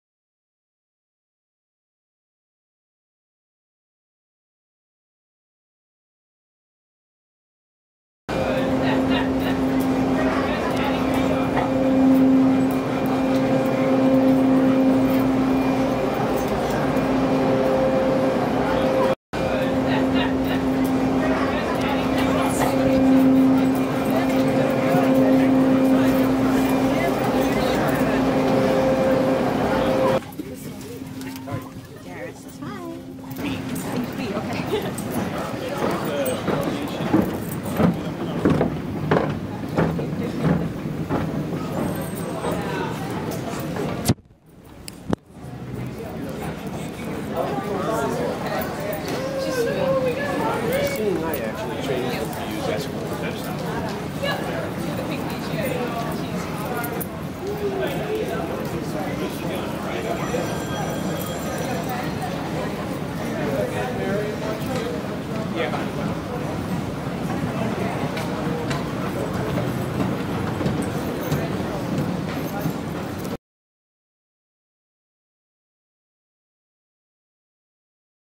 Vinson Goes to the Airport
Vinson the Seeing Eye puppy, and his fellow puppy pals, make a special visit to Newark Liberty Airport to expose the dogs to traveling experiences they will eventually share with their partner as a guide dog team.